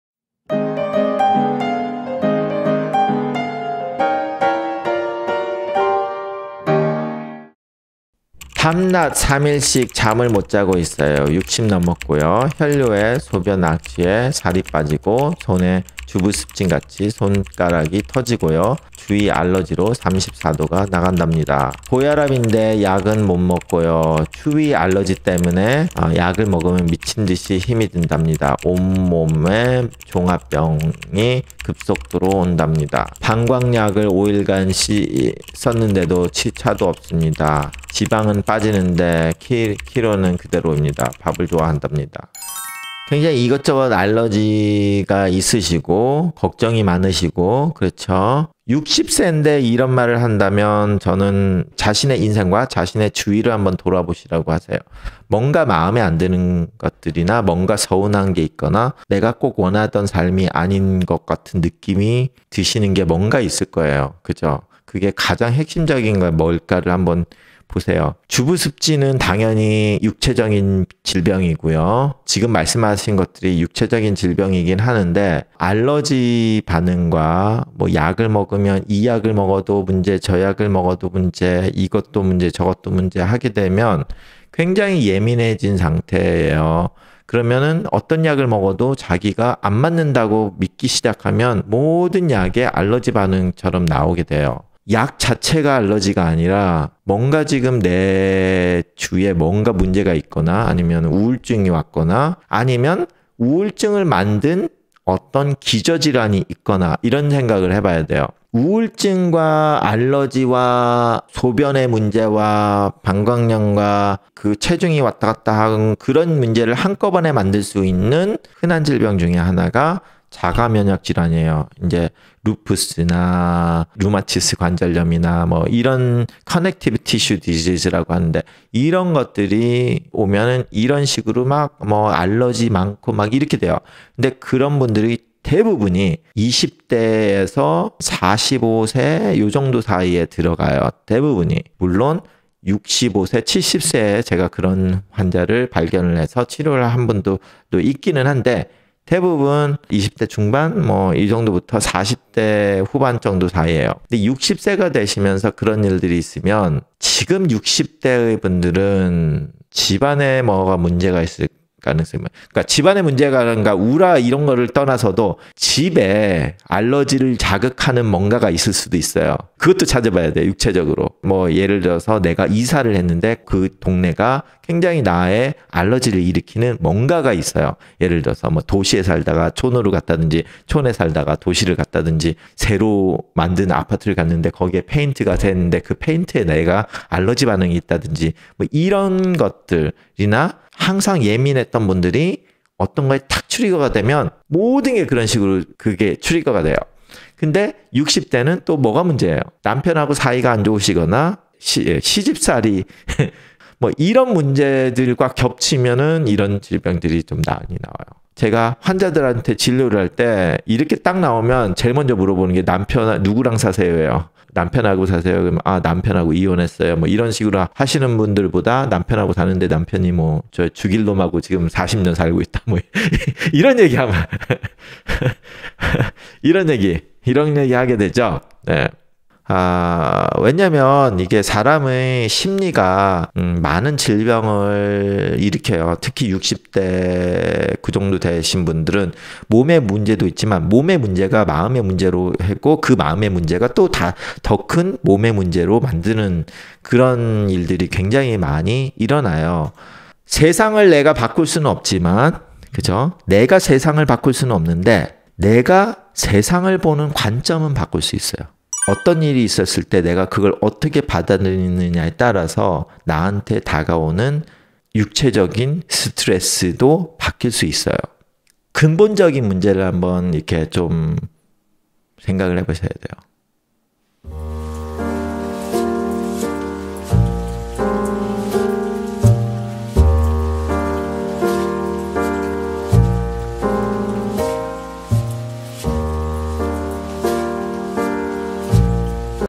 온몸이 다 아파요, 여러약에 알러지, 추위알러지, 스트레스받는 환자의 원인
[조동혁 내과/신장내과]